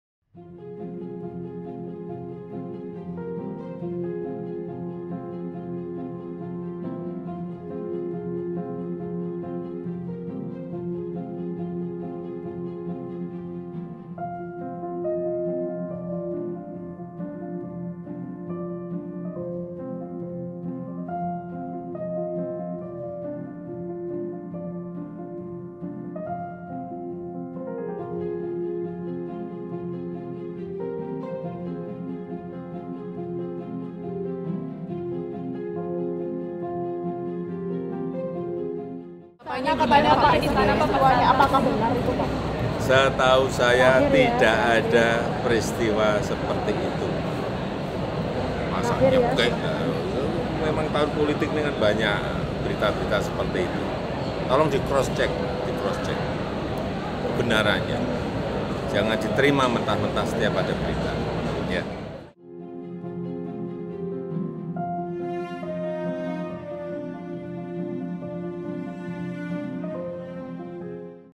Isu Prabowo Cekik Wamen, Jokowi: Masa Nyekek?
Hal ini disampaikan Jokowi usai meninjau Pasar Jatinegara Jakarta pada selasa pagi. Selain membantah, menurut Jokowi banyak isu miring yang muncul setiap tahun politik. Jokowi meminta agar selalau mengecek kebenaran akan  isu-isu yang bermunculan pada tahun politik.